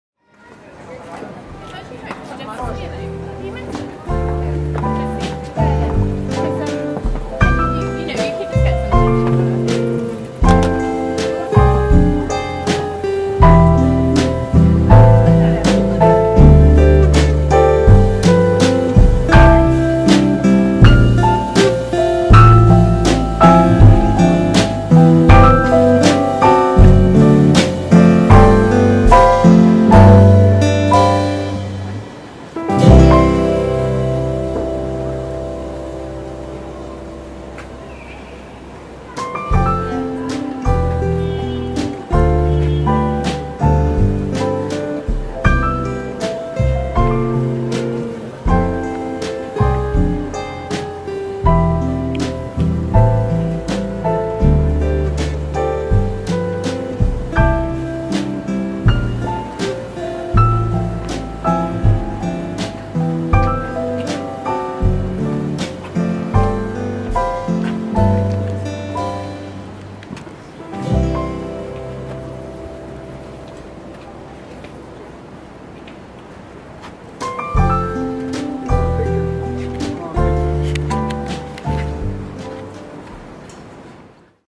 Girona, Catalonia /Game of Thrones movie new location/Жирона
Girona is a city in Spain’s northeastern Catalonia region, beside the River Onyar. It’s known for its medieval architecture, walled Old Quarter (Barri Vell) and the Roman remains of the Força Vella fortress. It’s vibrant nightlife scene centers on the Plaça de la Independència and Plaça de Sant Feliu, and its restaurants are renowned for both traditional Catalan and cutting-edge cuisine.